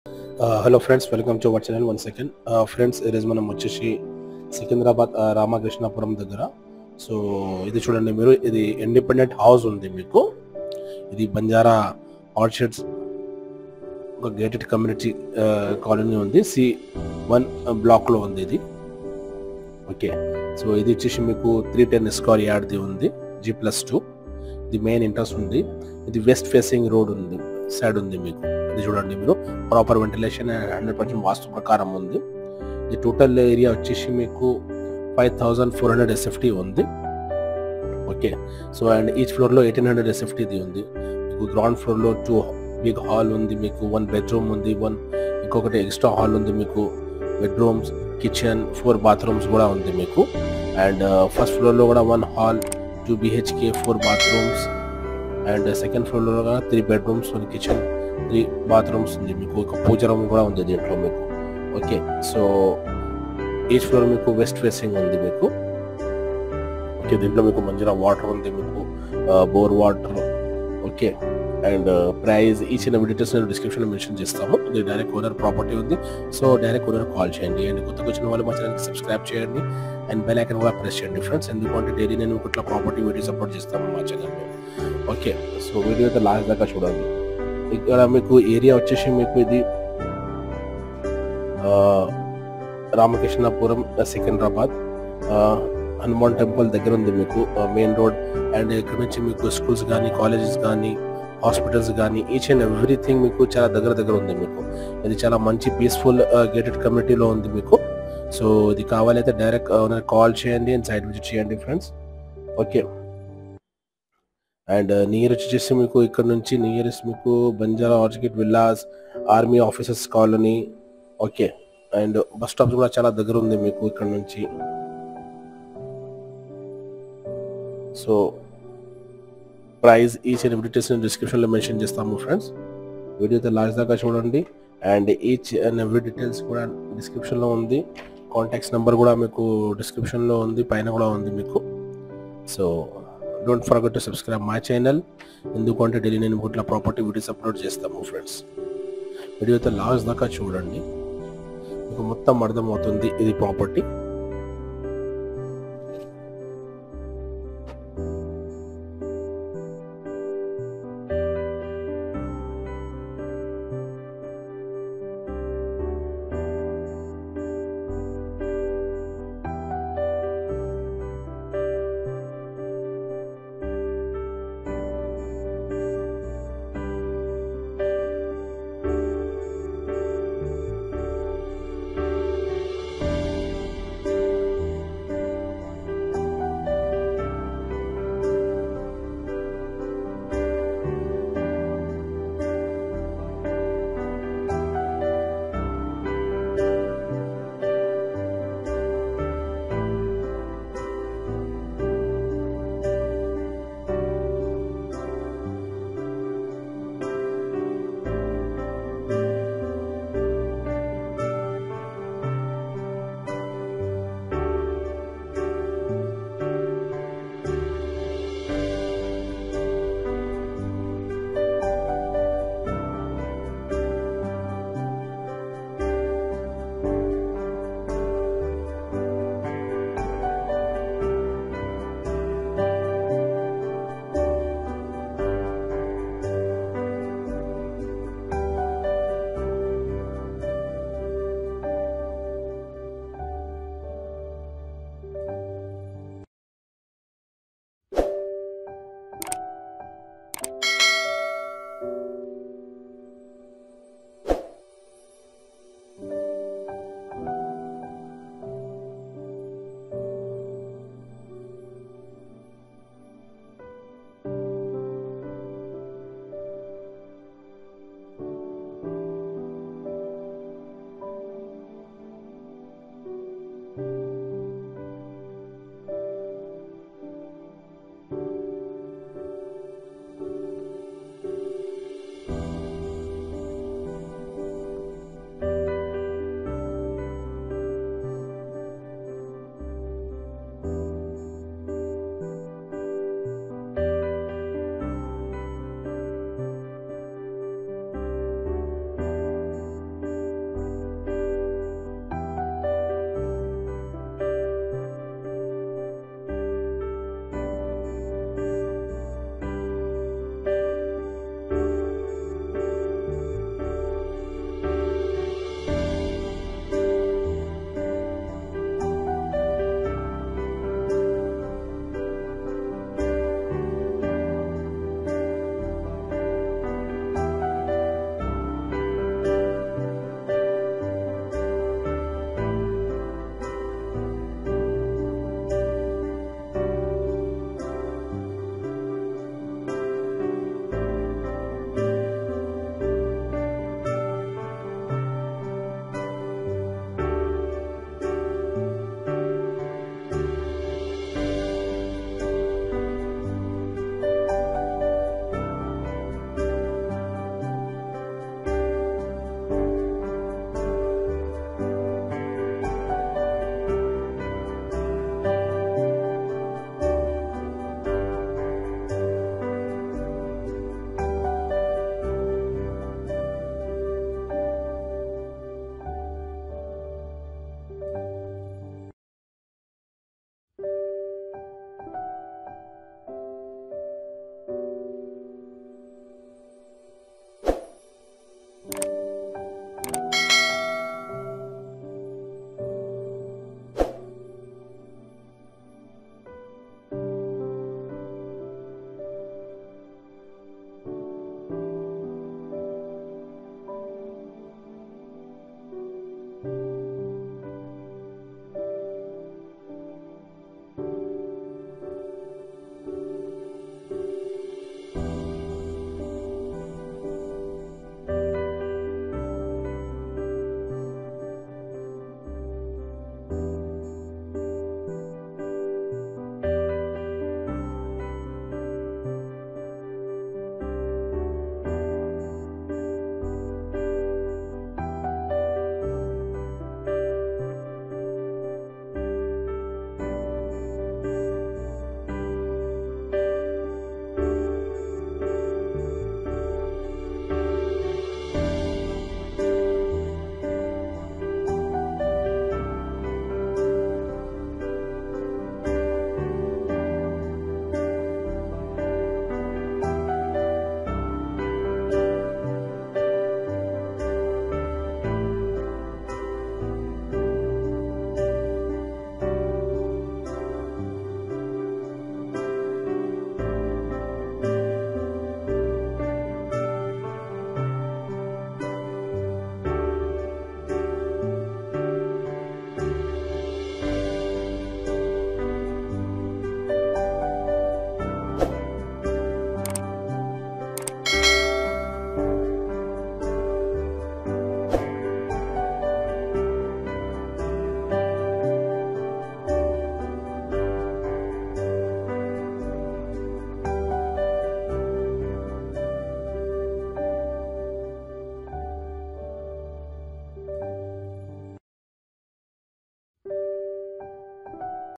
House for sale at @SECUNDERABAD : 9849094728.DO YOU WANT TO SELL/RENT YOUR PROPERTIES: 9949092140.
WEST FACE G+2 HOUSE SALE 310 SQYD HOUSE SALE @ SECUNDERABAD  RAMKRISHNAPURAM  NEAR HANUMAN TEMPLE  Near Orched Villas
5400 SFT EACH FLOOR 1800 SFT
ON GROUND FLOOR 2 HALL 1 BESROOM AND I HALL 4 BATHROOMS
1ST FLOOR 1 HALL 2BHK AND 4 BATHROOMS
2ND FLOOR 3 BEDROOMS 1 ITCHEN 3 BATHROOMS AND POOJA ROOM
MANJEERA WATER , BORE WATER  AND ALL AMINITES AVILABLE
BANK LOAN AVILABEL  PRICE 2.5 Cr Neg …